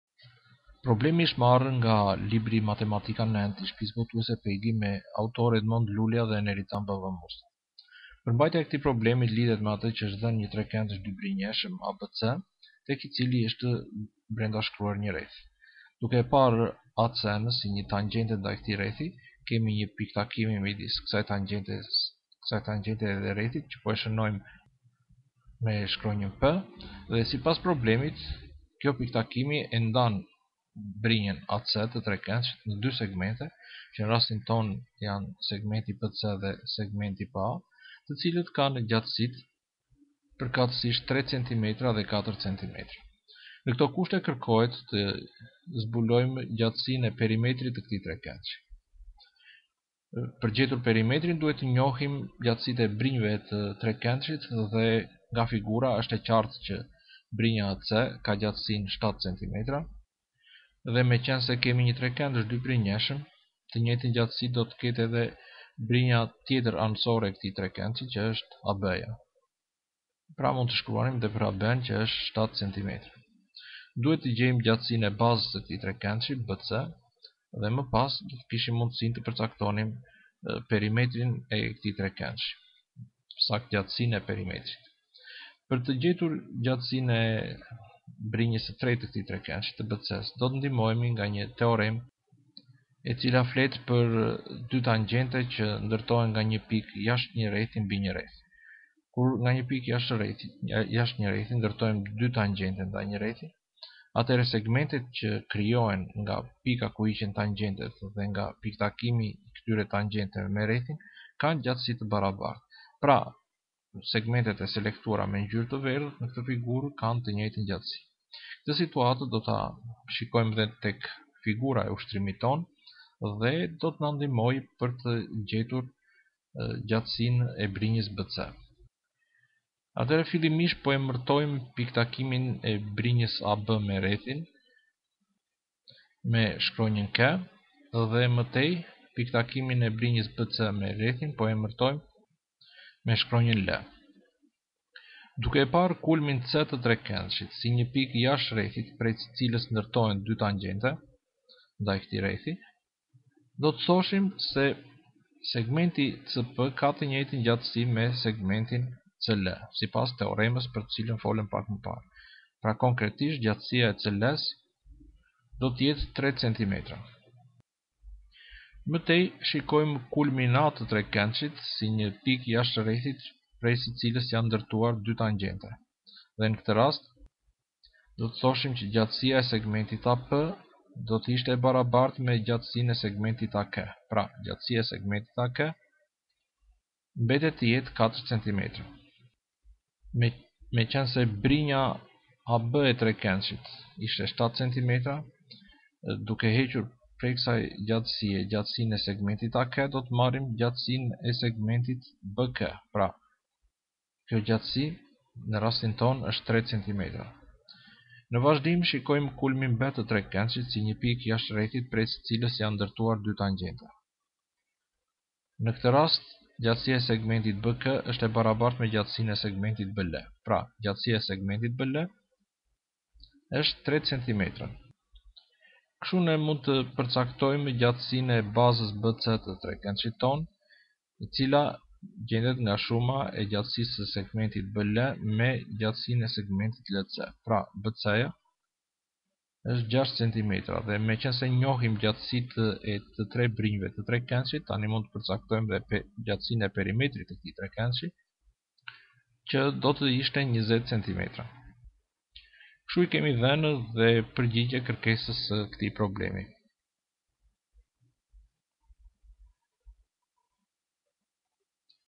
Matematika 9 (Pegi) 5.5/7
Video të tjera dhe të organizuara do të gjeni tek linku:
Ushtrimi është shkëputur nga libri Matematika 9 (Pegi) me autorë Edmond Lulja dhe Neritan Babamusta. Ushtrimi i takon kapitullit 5 (Njohuri plotësuese për rrethin), mësimi 5.5 (Rrethi i brendashkruar trekëndëshit), ushtrimi 7.